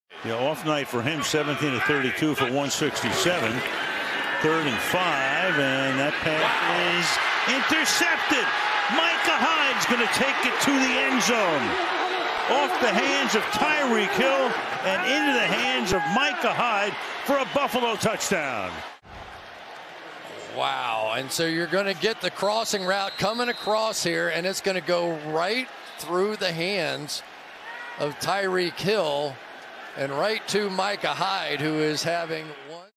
Buffalo Bills Touchdown of the Day for November 12, 2024
Micah Hyde goes 26 yards with a Patrick Mahomes Pass in the 3rd quarter of an October 2021 game.